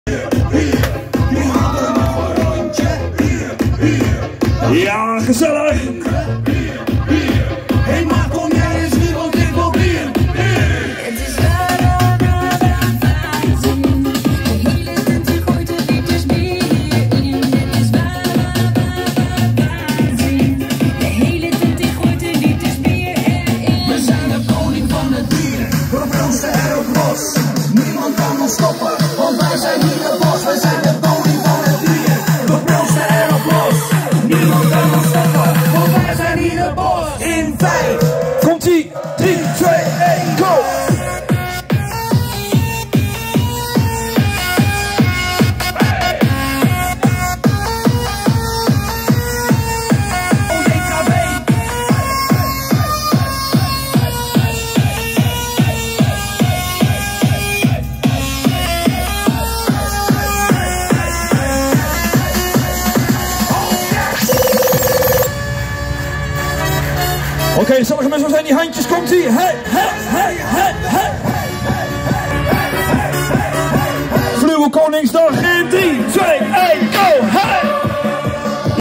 Koningsdag 2021 in Hooge Zwaluwe
Zoals altijd was het een feestje in Hooge Zwaluwe op Koningsdag. Dit jaar zelfs met een Flügel-feestarrangement die Prinses Margrietstraat-bewoonster Elise van Geel gewonnen had. Wij waren erbij, maakten deze opname en foto's die u kunt bekijken